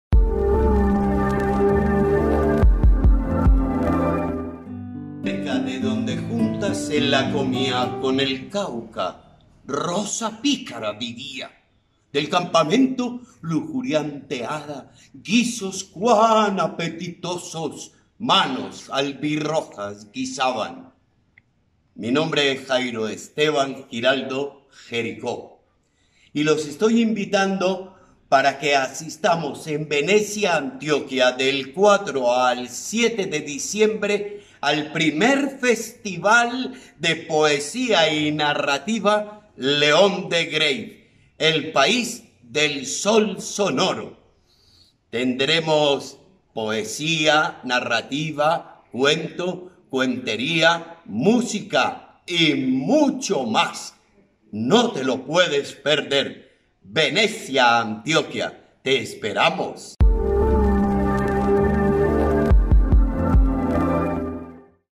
1er Festival de Poesía y Narrativa León de Greiff - País del Sol Sonoro
🎈¡Se acerca!
☀️¡Los esperamos en Venecia, la de Antioquia! Programación en área urbana y en nuestro corregimiento de Bolombolo, donde vivió el poeta entre 1926 y 1928 y laboró como contabilista del Ferrocarril 🛤 de Antioquia.